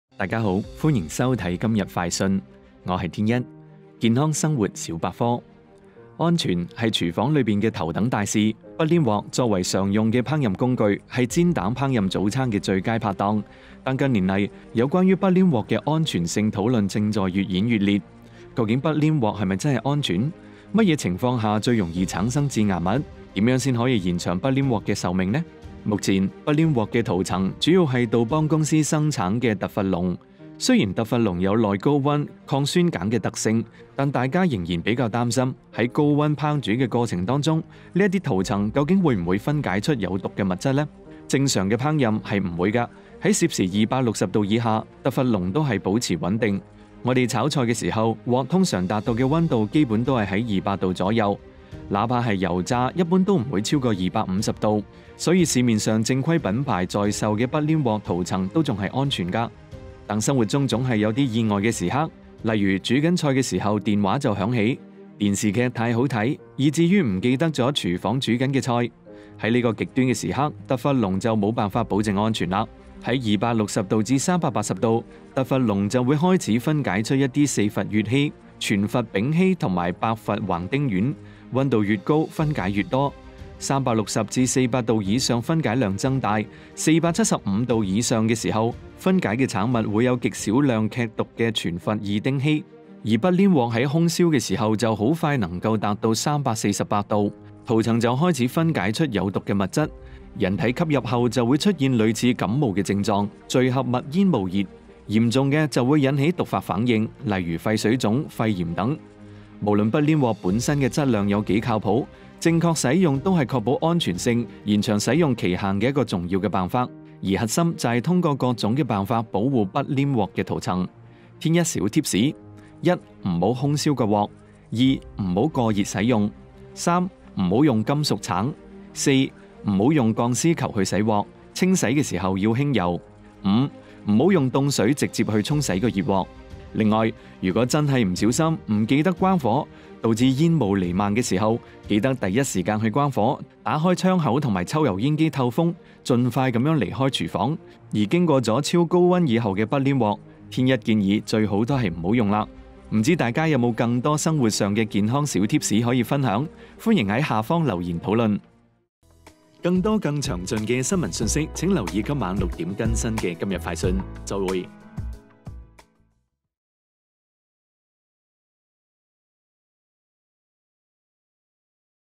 今日快訊：不粘鍋真的安全嗎 ？ 什麼情況下容易產生致癌物 ？
粵語|今日快訊：不粘鍋真的安全嗎 ？ 什麼情況下容易產生致癌物 ？

健康生活小百科
（不粘鍋真的安全嗎 ？ 什麼情況下容易產生致癌物 ？）
安全，是廚房裡的頭等大事。不粘鍋作為常用的烹飪工具，是煎蛋烹飪早餐的最佳拍檔。但近些年來， 有關於不粘鍋安全性的討論正愈演愈烈 。
那不粘鍋真的安全嗎 ？ 什麼情況下容易產生致癌物 ？ 如何延長不粘鍋的壽命呢 ？
不粘鍋的塗層安全嗎？
目前不粘鍋的塗層主要是杜邦公司生產的特氟龍（主要為聚四氟乙烯）。雖然特氟龍有耐高溫、抗酸鹼的特性，但大家仍比較擔心，在高溫的烹飪過程中，這種塗層會不會分解出有害物質。正常烹飪的話是不會的。在260℃以下，特氟龍都能保持穩定 。
我們炒菜時鍋具能達到的溫度基本都在200℃左右，哪怕是油炸爆炒一般也不會超過250℃，所以市面上正規品牌在售的不粘鍋的塗層還是安全的。生活中總有些意外時刻，剛燒上鍋電話響了、電視劇太好看以至於把廚房裡煮著的菜給忘了……
在這些極端時刻，特氟龍也沒辦法保證安全了！
在 260-380℃ ，特氟龍就會開始分解出一些四氟乙烯、全氟丙烯和八氟環丁烷， 溫度越高，分解越多 ，3 60-400℃以上分解量增大 ， 475℃ 以上時 ， 分解產物會有極少量劇毒的全氟異丁烯 。而不粘鍋空燒幾分鐘就可達到348℃，塗層即開始分解出有害物質。 人體吸入後會出現類似流感症狀的“聚合物煙霧熱”，嚴重的會引起毒性反應，如肺水腫、肺炎等 。
無論不粘鍋本身的質量有多靠譜，正確使用都是確保安全性、延長安全使用期限的一個重要辦法 。核心是通過各種方法保護不粘鍋的塗層，天一小Tips:
❌ 不要空燒鍋；❌ 不要過熱使用；❌ 不要用金屬鏟；❌ 不要用鋼絲球等洗鍋，清洗時要輕柔；❌ 不要用冷水直接沖洗熱鍋。
另外，如果真的不小心忘記關火導致煙霧瀰漫的時候，記得第一時間關火，打開窗口和抽油煙機，並儘快廚房，不要呆在煙霧裡！而經歷過超高溫後的不粘鍋，天一建議最好還是不要用了，不知道大家有沒有更多生活健康小Tips可以分享， 歡迎在下方留言討論！